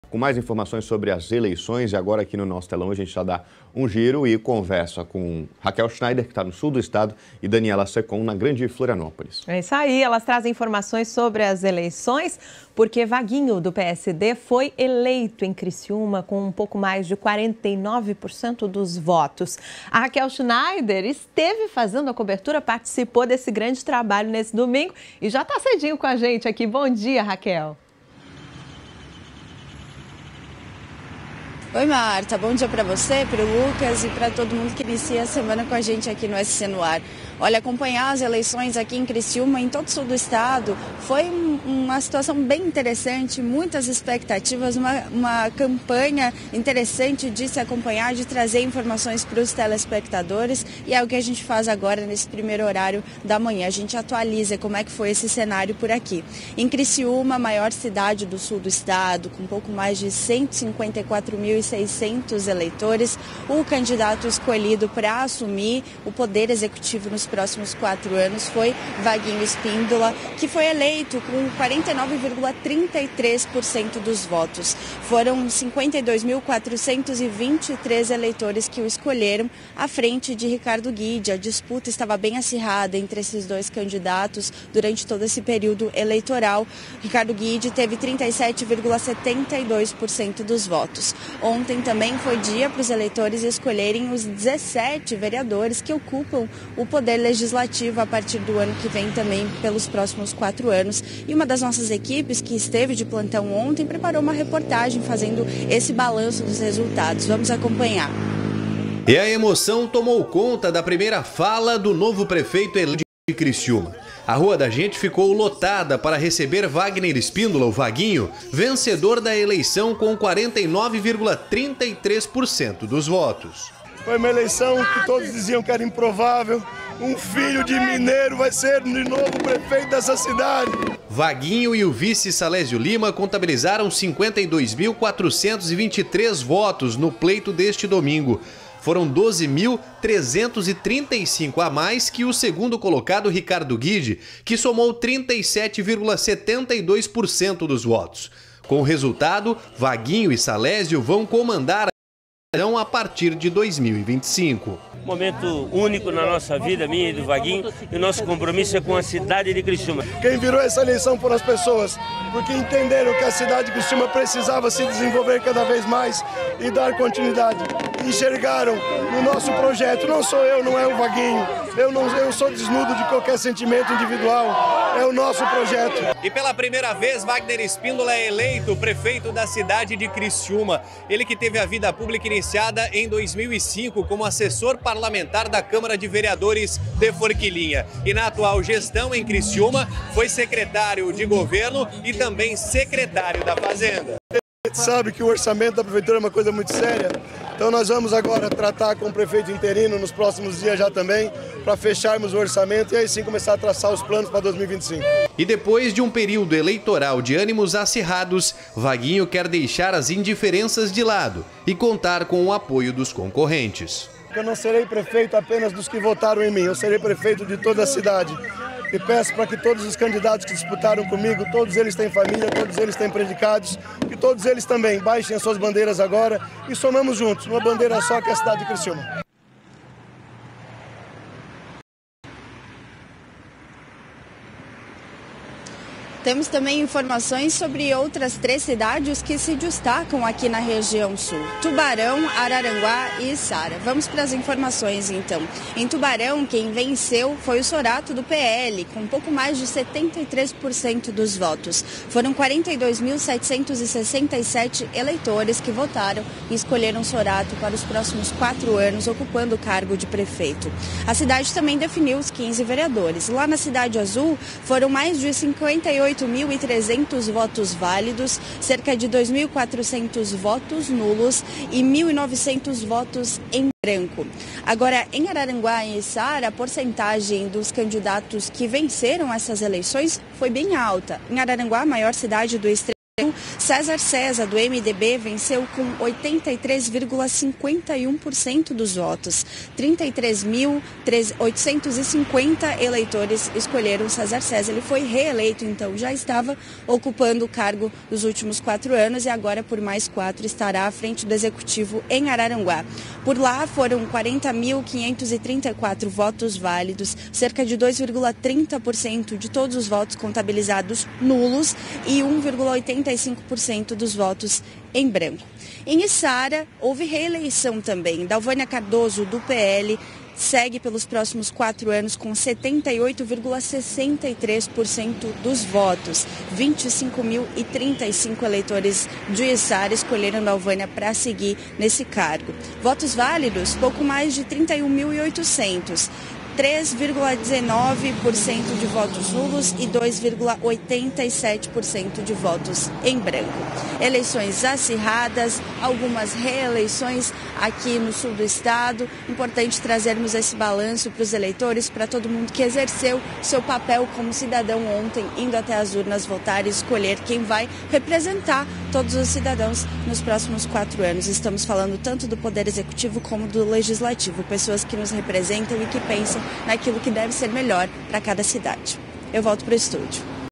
Eleições municipais: Vaguinho do PSD é o novo prefeito de Criciúma
Na mais recente eleição municipal, Vaguinho, do PSD, elegeu o cargo de prefeito de Criciúma com pouco mais de 49% dos votos. A vitória é um reflexo da confiança da população em sua liderança e propostas. Além disso, outros resultados importantes foram registrados na região: Soratto, do PL, foi eleito prefeito de Tubarão, César Cesa, do MDB, aposentou-se na prefeitura de Araranguá, e Dalvania Cardoso, do PL, tornou-se a primeira prefeita de Içara. Acompanhe nossa cobertura ao vivo com Rachel Schneider, que traz todos os detalhes sobre as eleições e o impacto dessas escolhas para as comunidades.